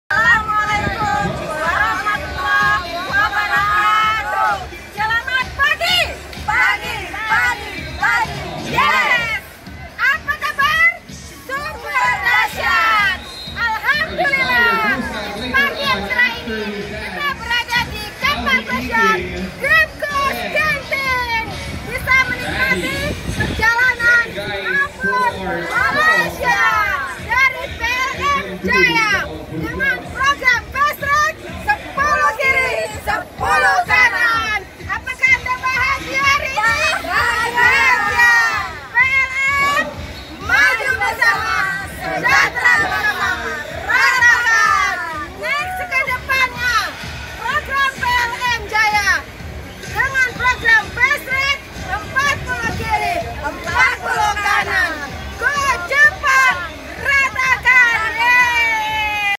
Ibu2 Leader PLM JAYA.. Berbahagia. Sangat Bahagia . Diatas kapal Pesiar Mewah Dream Cruise..
Luaar Biasaa.
PLM JAYA.
Super Dahsyaat.
Maju Bersama.
Sejahtra Bersama.
Rrrrraaaattaakaann.
Sangat.. Bahagia .
Selalu Bahagiaa.